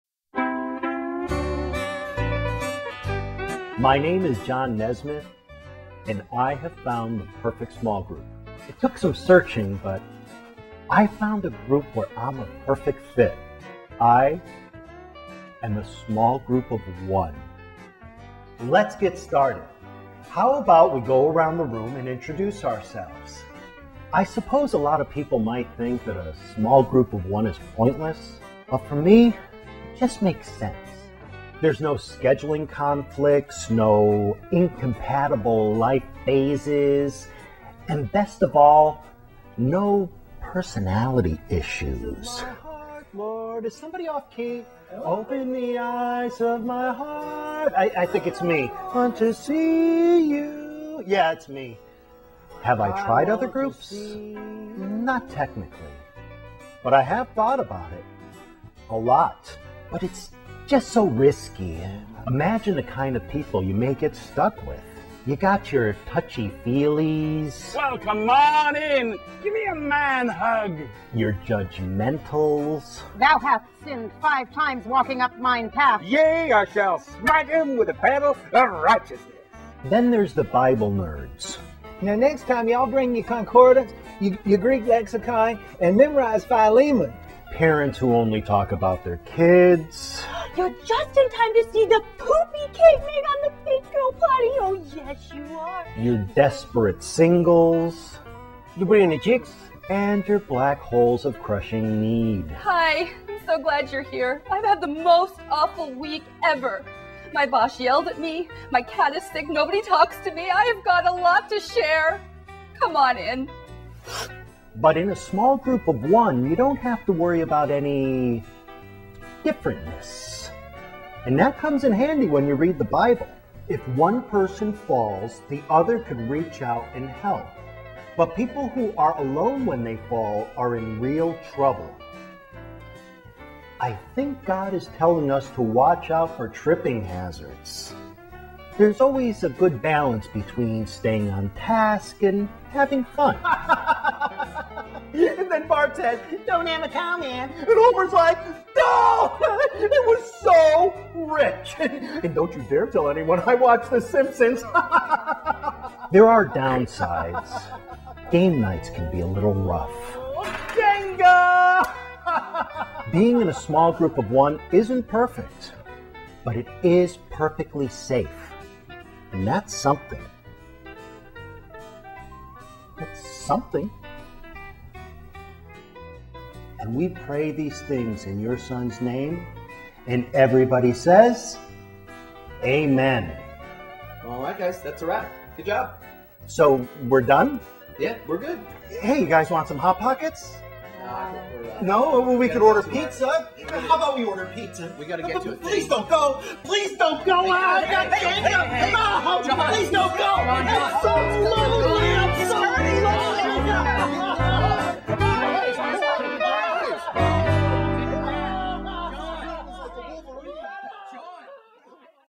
Small Group of One
A man finds the perfect small group. Himself. Based on a brilliant original concept by Vintage 21.